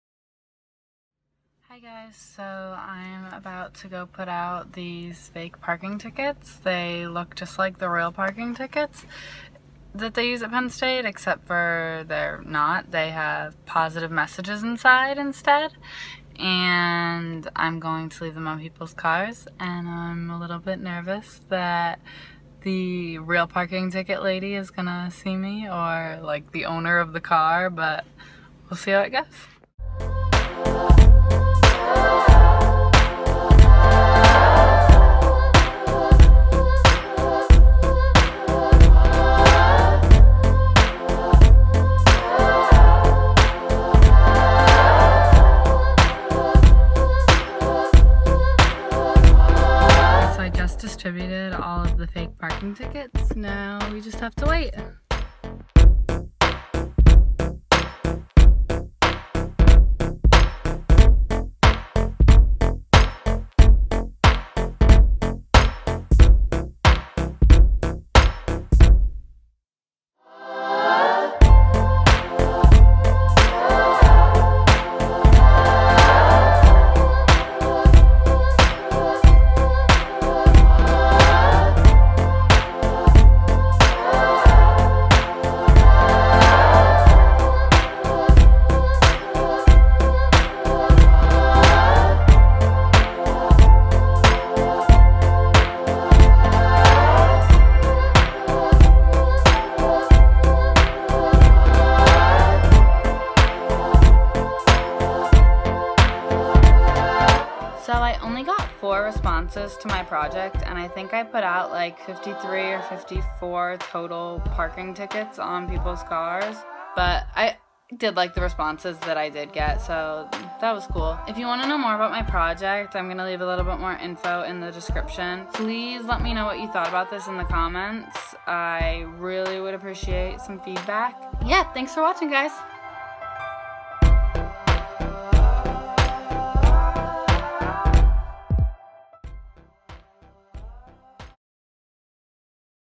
ProjectHappinessPSU
This project was for one of my art education classes as part of a "culture jamming" assignment. If you don't know what culture jamming means, you can read about
I want to hear your thoughts before I tell you what I was trying to achieve but I promise I will tell you later.